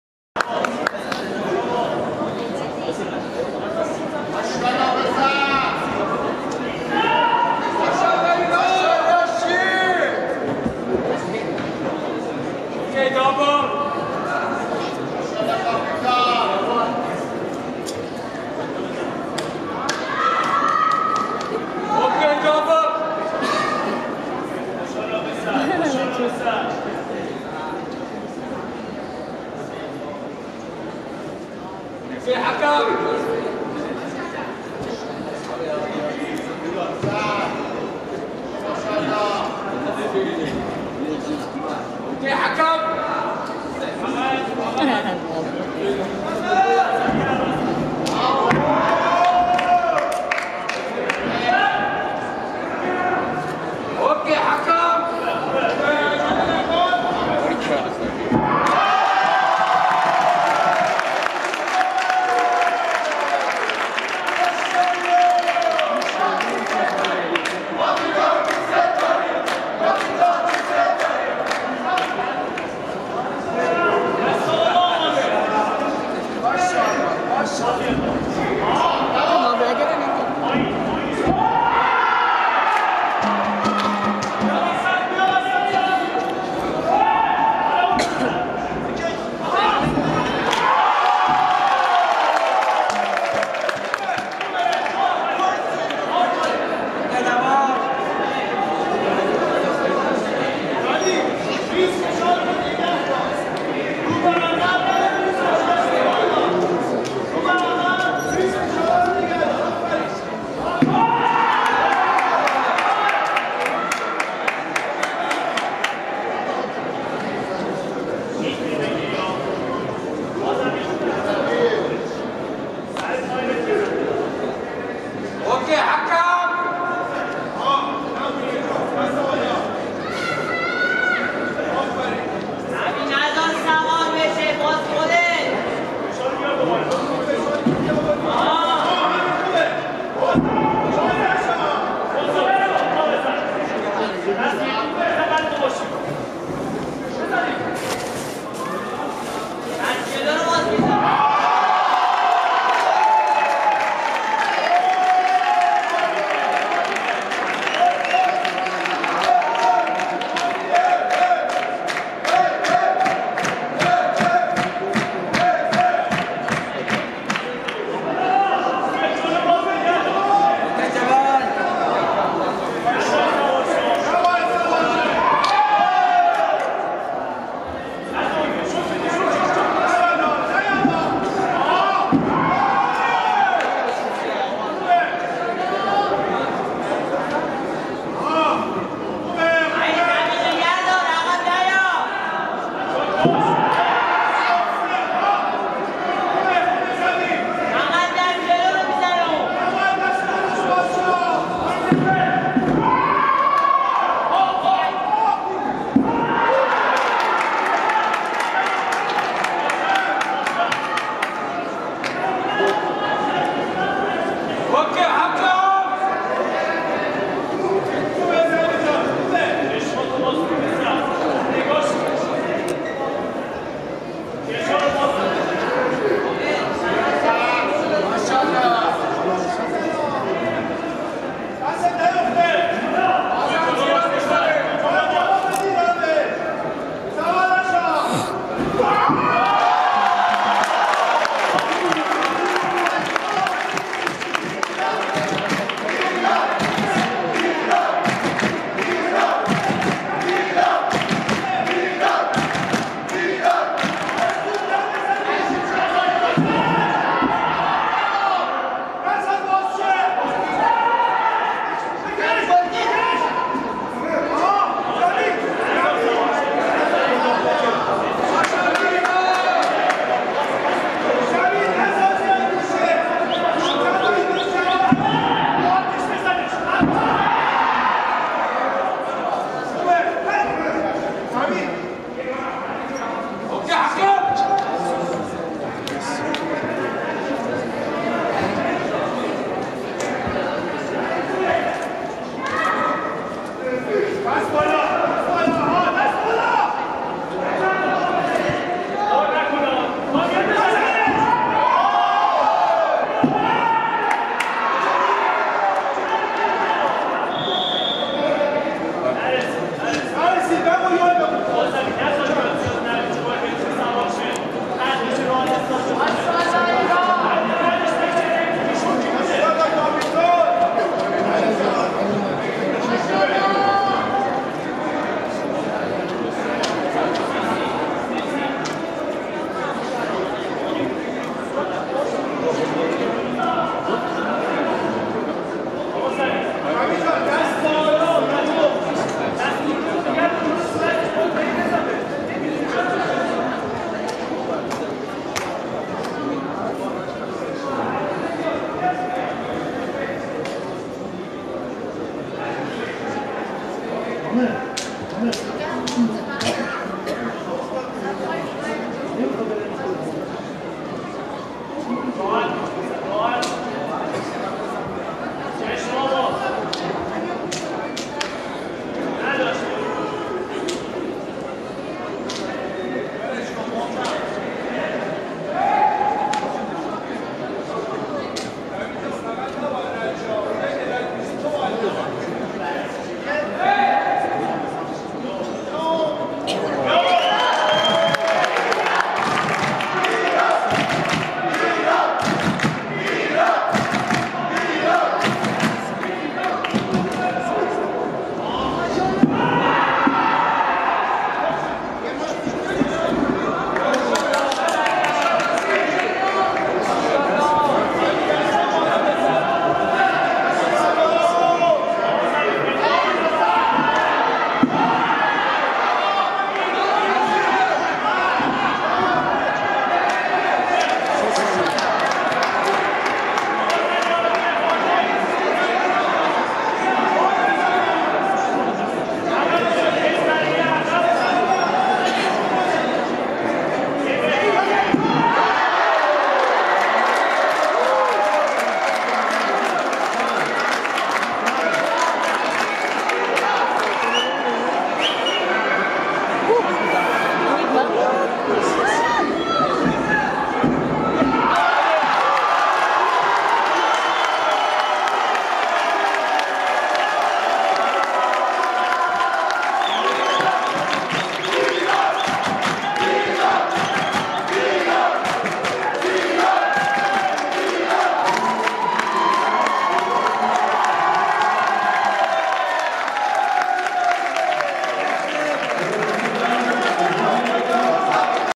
FINAL Zabiollah Poorshab (IRAN) Vs Shimada Rikito (JAPAN) AKF Championship 2021 Almaty Kazakhstan
AKF Championship 2021 Almaty, Kazakhstan 18-22 December 2021

Send your MAIL for MAILTIME to
Ricky Muchtar

OSH